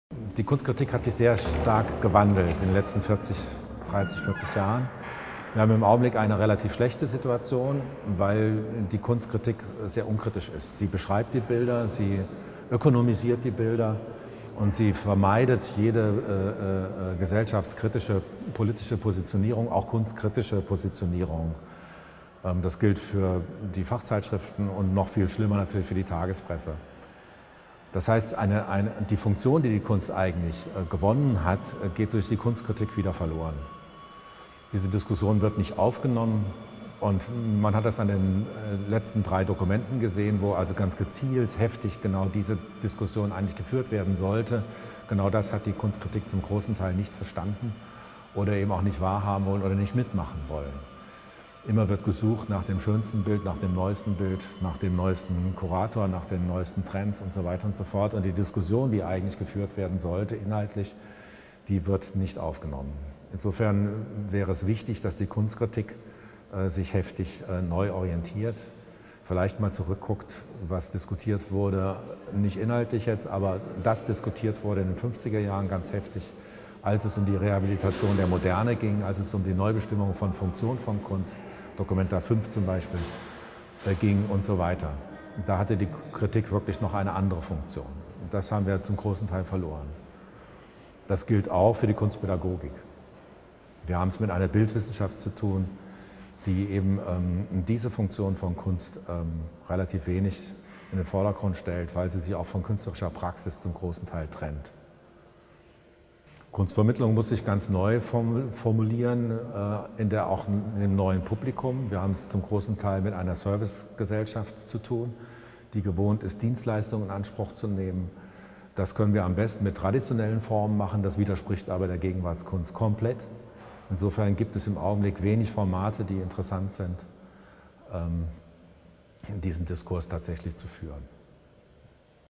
Kunstkritik, Kunstpädagogik und Kunstvermittlung - Bernhard Balkenhol
Bernhard Balkenhol, Vorsitzender/Kurator des Kasseler Kunstvereins, über Kritik, Pädagogik und Vermittlung in der Gegenwartskunst.

Seminar an der Akademie der Bildenden Künste München zur Vermittlung von Gegenwartskunst im Kontext der dOKUMENTA (13)

Kamera, Redaktion: Alexander Eisfeld